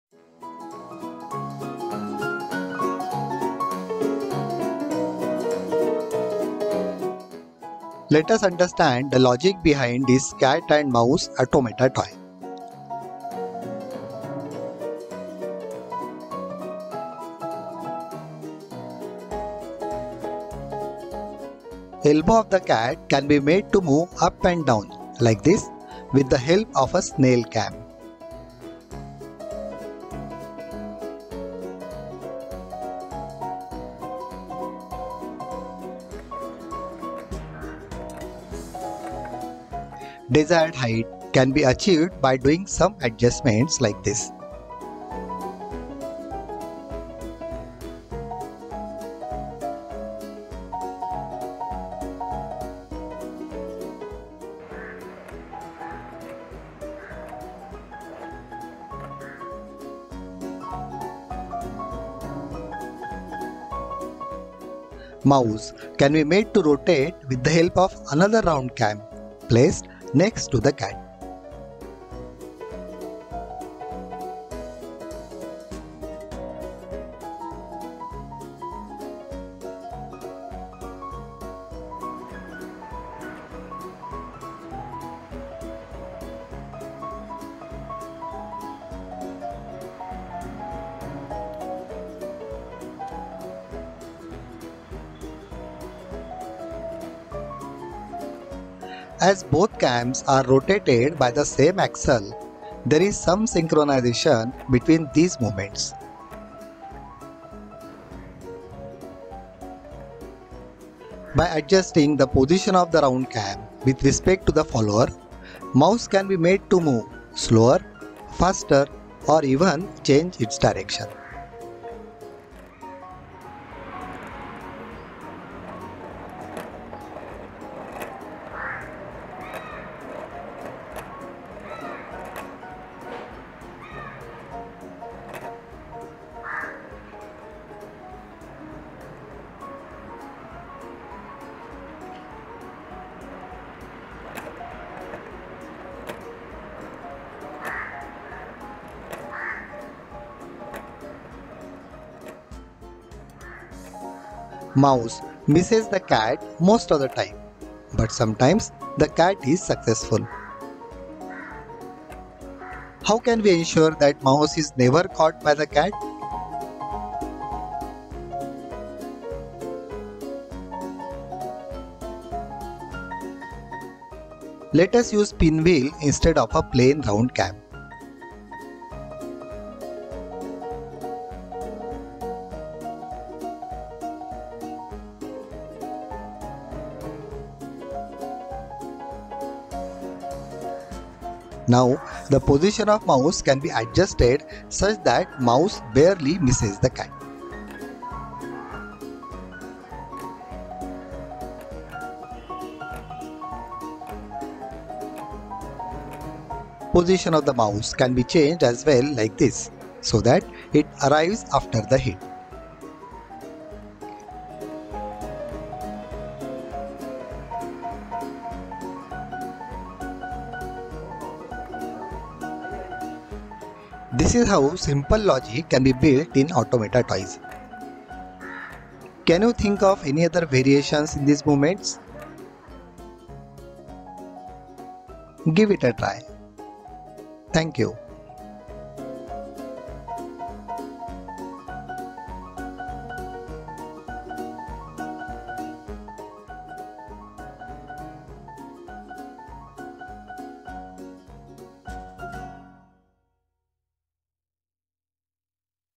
Cat and Mouse - Simple Logic in Automata Toys
Simple logic explained with the help of snail cam, round cam and pinwheels.